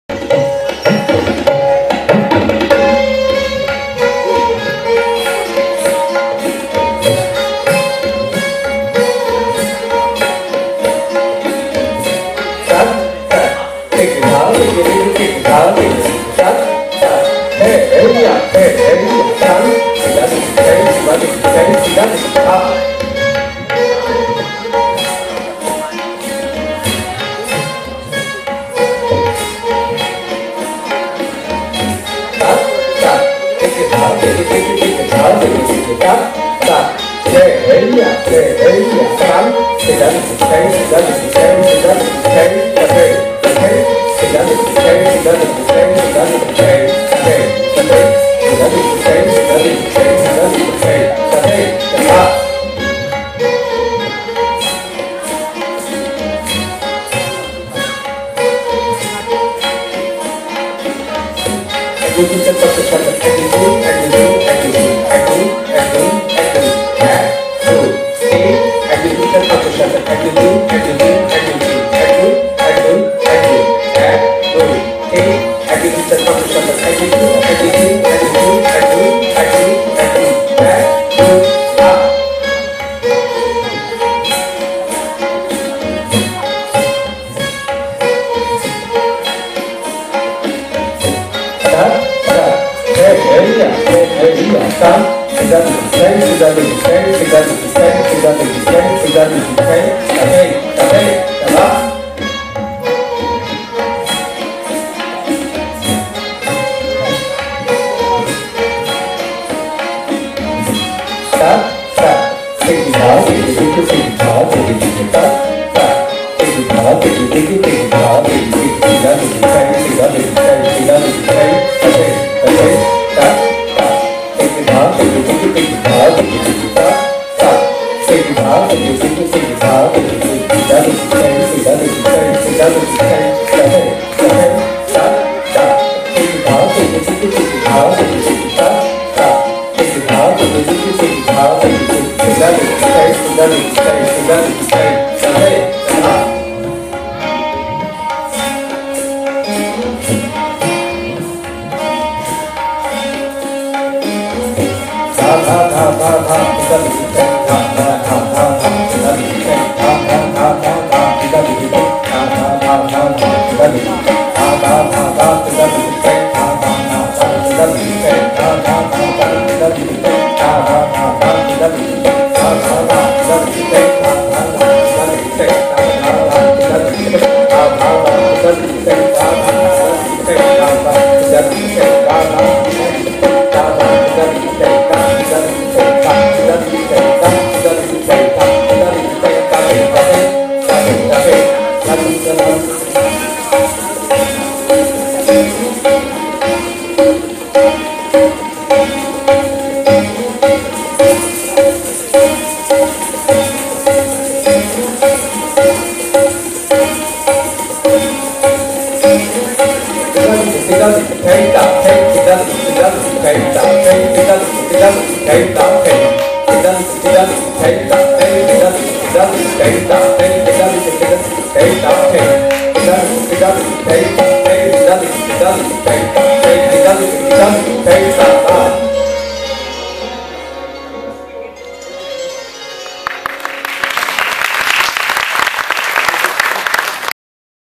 Katthak group
A group katthak performance by Sinchon, Onty, Kongka, students of Gontabbya. This classical dance was performed on the stage of Katthak Dance Festival, dhaka, 2014.
Choreographed by- Saju Ahmed